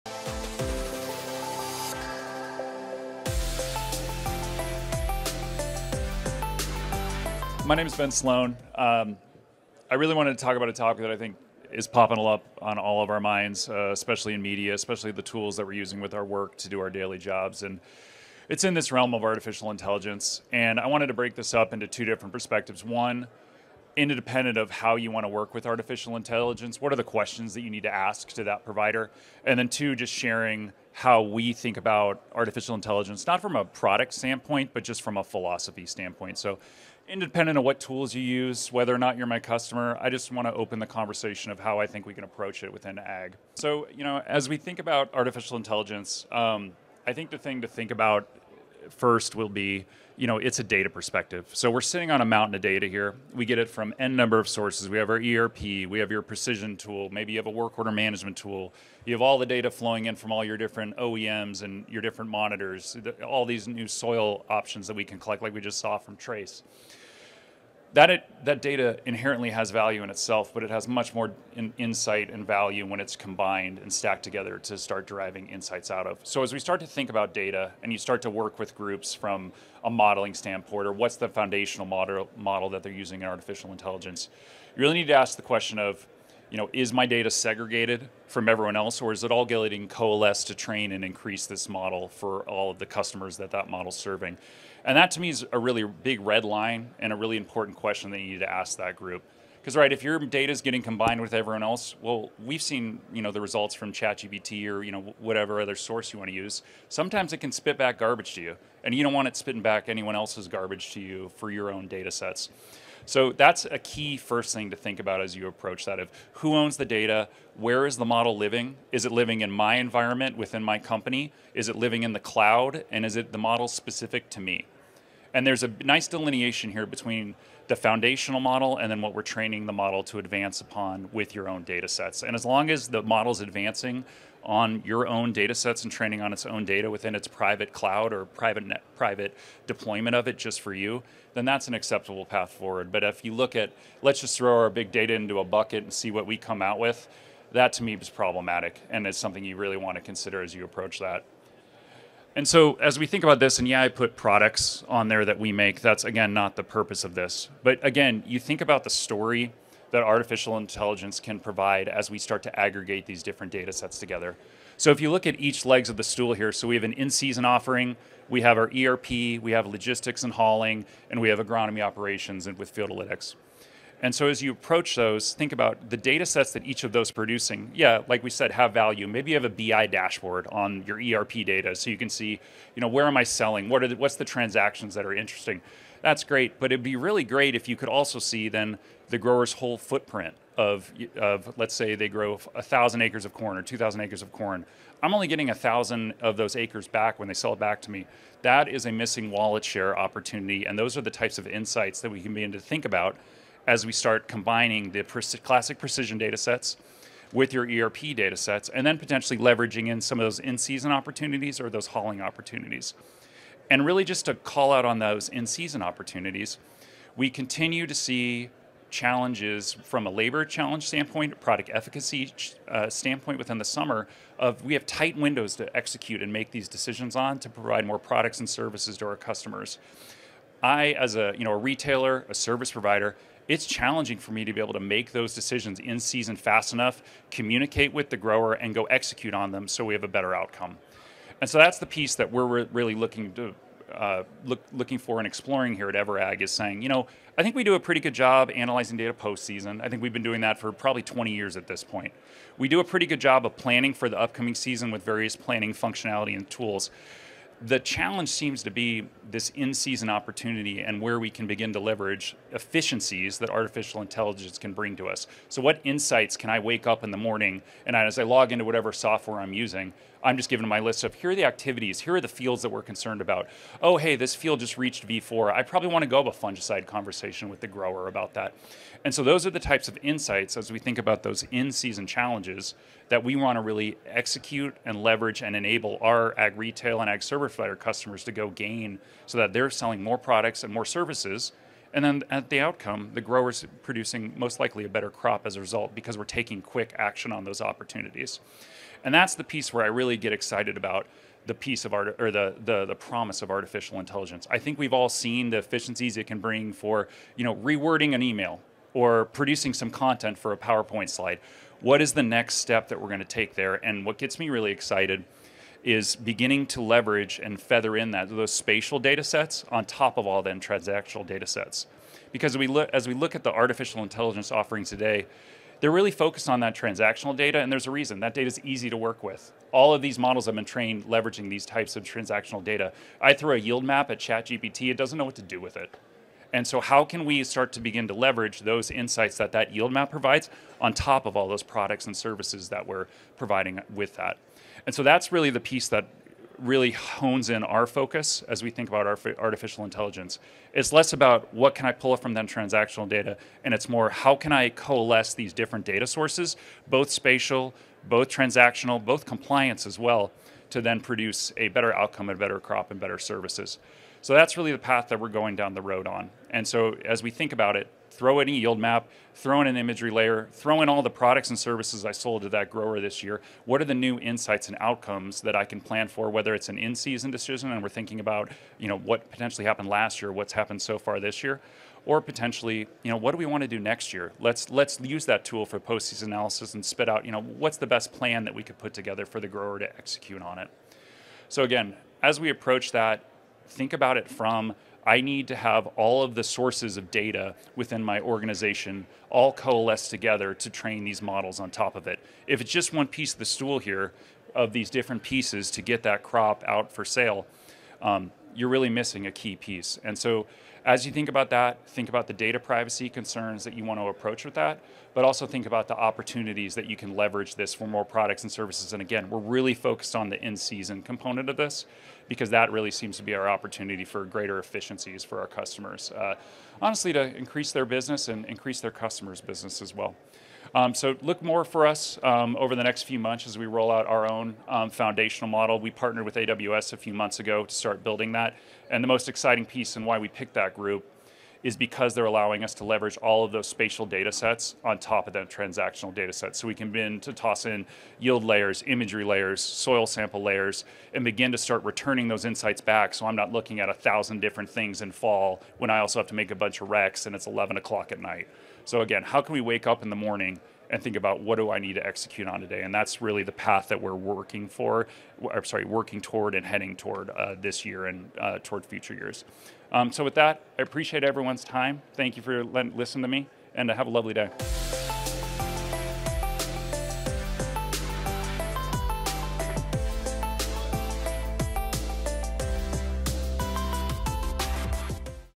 Leveraging Artificial Intelligence for Ag Retail
Artificial intelligence has exploded over the past year but its applications have struggled to bring value to agronomic advisors. Learn about how ag service providers can approach artificial intelligence and make their business more efficient.